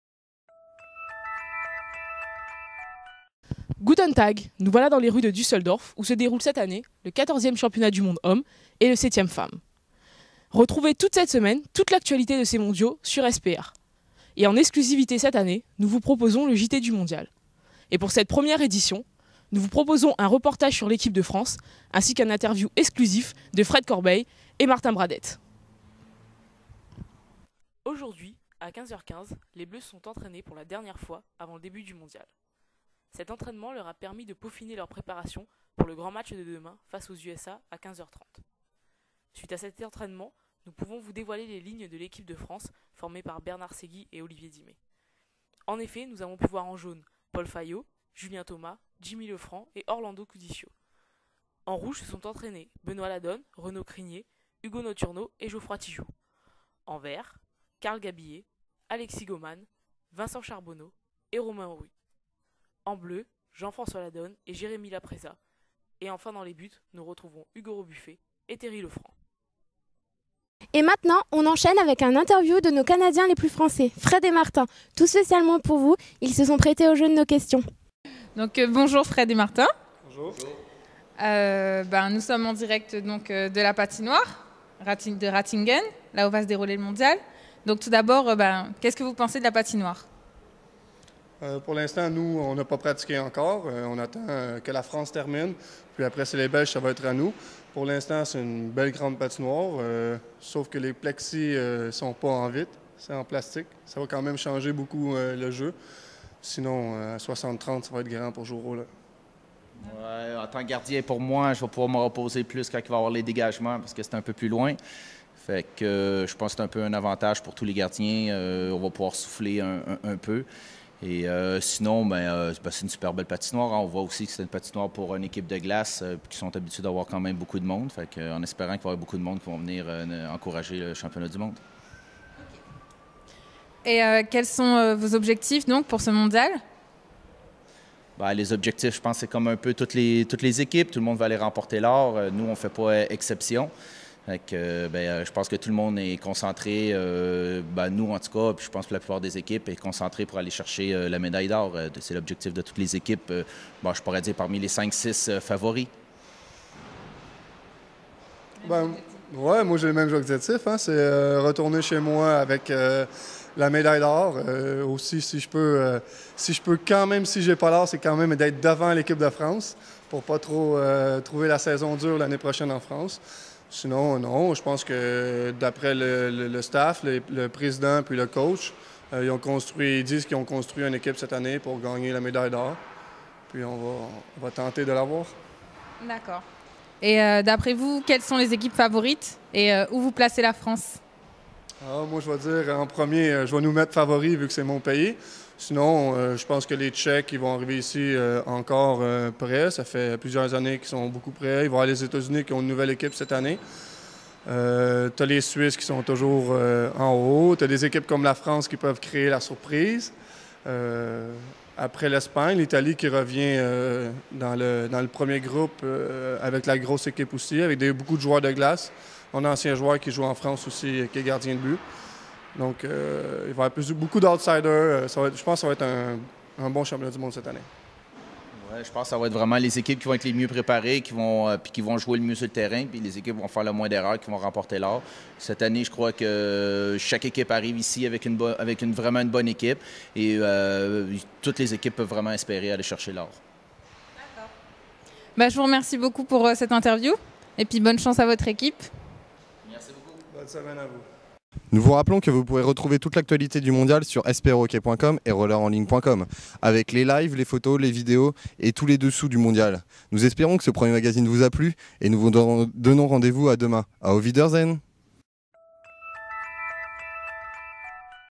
Vidéo SPR - Mondial 08 Dusseldorf : Jt du Mondial N°1 du 05/07/08
Retrouvez le premier Journal Télé du mondial 2008 de roller Hockey qui se déroulé à Dusseldorf en Allemagne - Edition du 05 Juillet 2008.